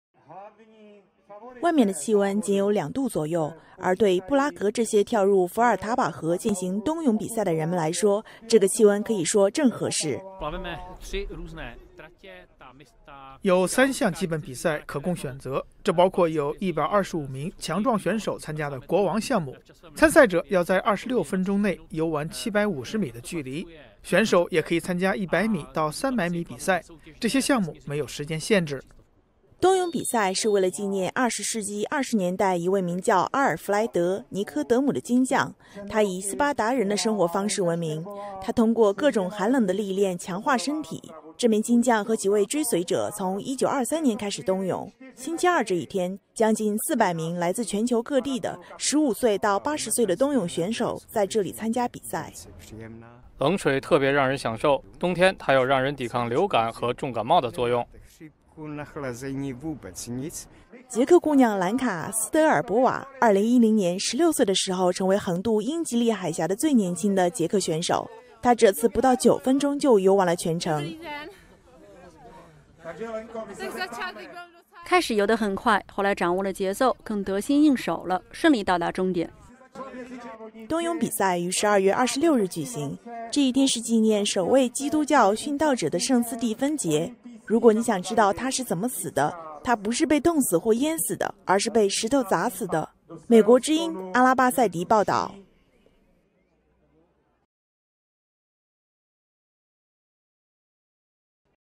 数百捷克人冬泳 庆祝近百年传统
星期二数百人跳入捷克共和国最长的河流进行每年一度传统的冬泳比赛， 水温只有4摄氏度。这项传统由一位金匠自1920年代开创，他以洗冷水澡和游冬泳等所谓“斯巴达人”的生活方式闻名。